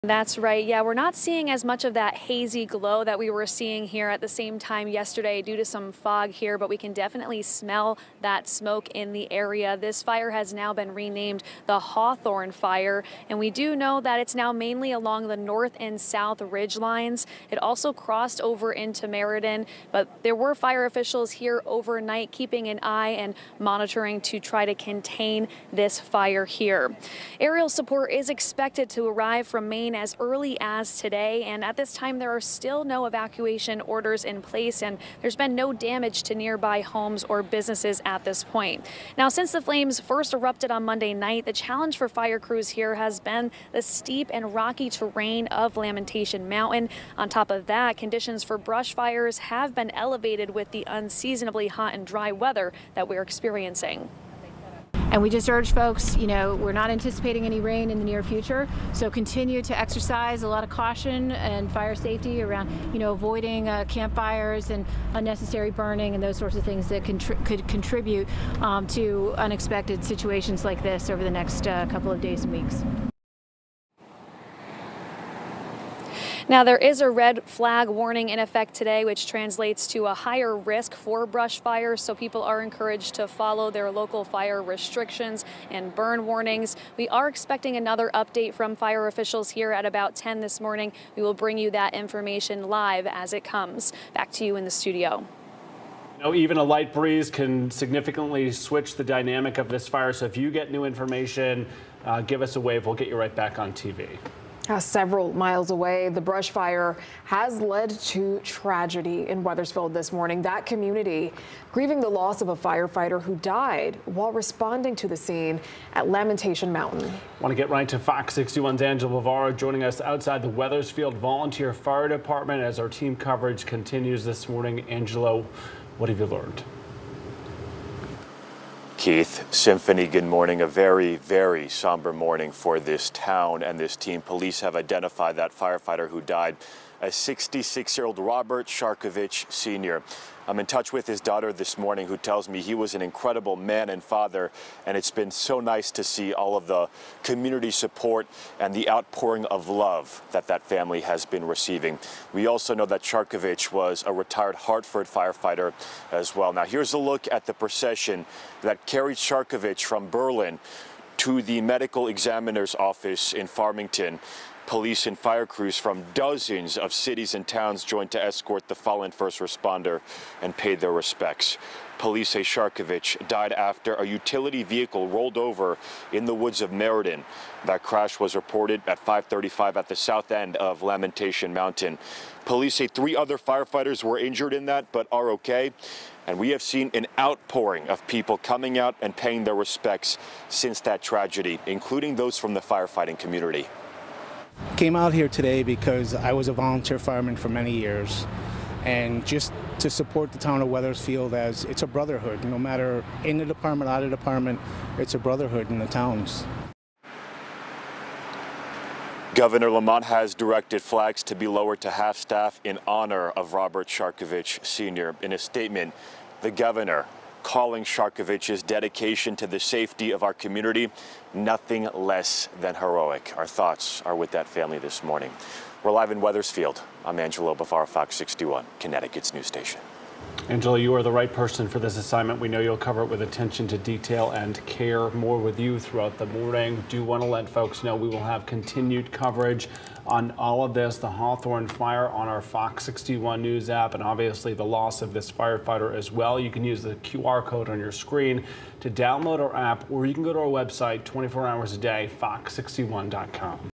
Fire crews honor fallen Wethersfield firefighter while continuing to battle Lamentation Mountain bla
Wethersfield volunteer firefighter 66-year-old Robert Sharkevich, Sr., was killed in the line of duty Tuesday while helping to battle a brush fire in Berlin.